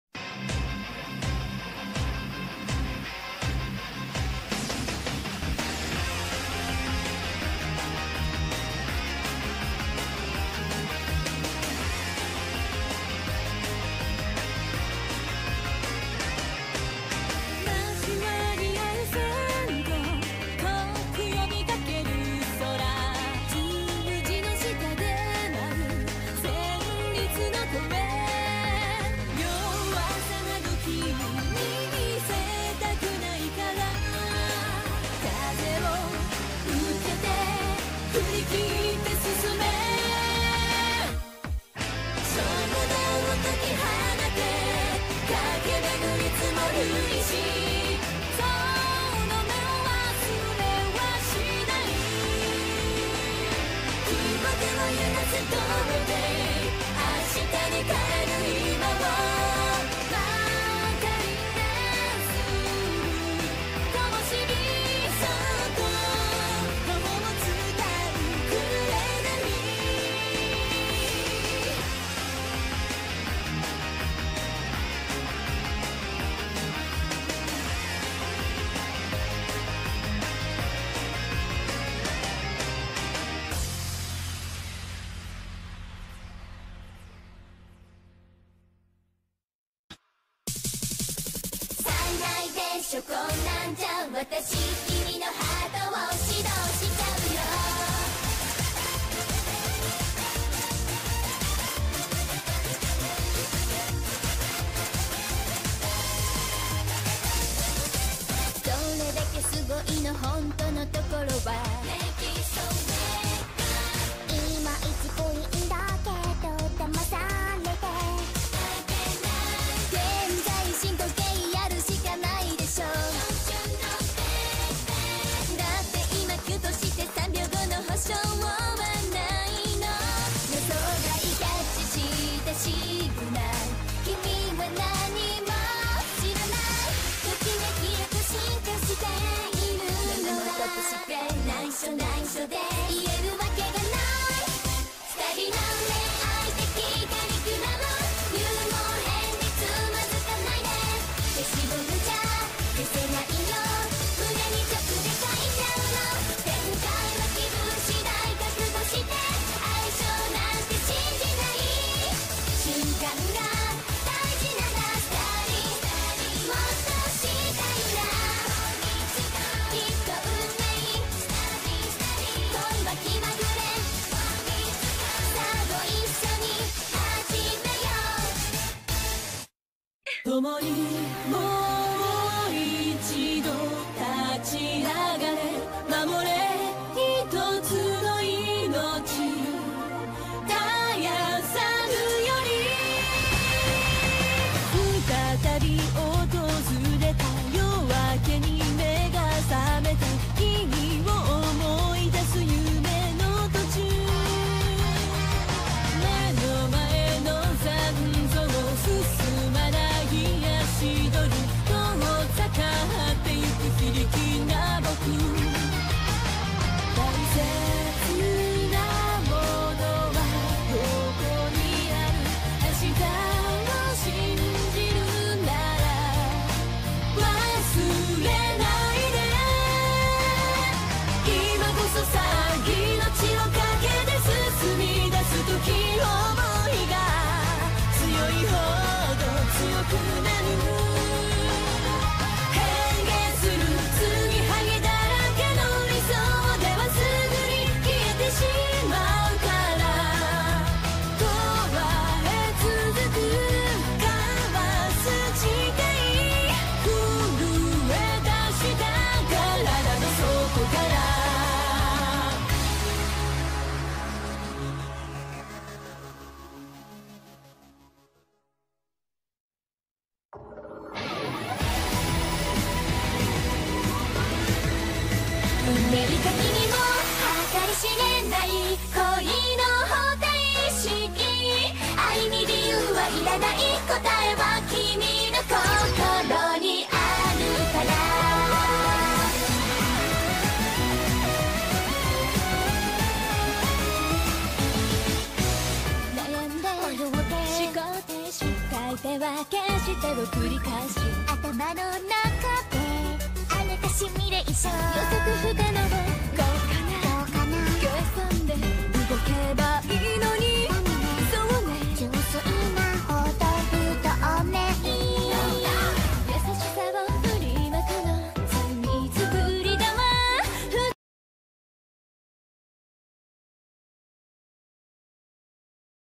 High School DxD Rossweisse
Hyperlapse video of me finishing of my artwork of Rossweisse from High School DxD.
A4 prints are available £5 each or for a digital copy £3.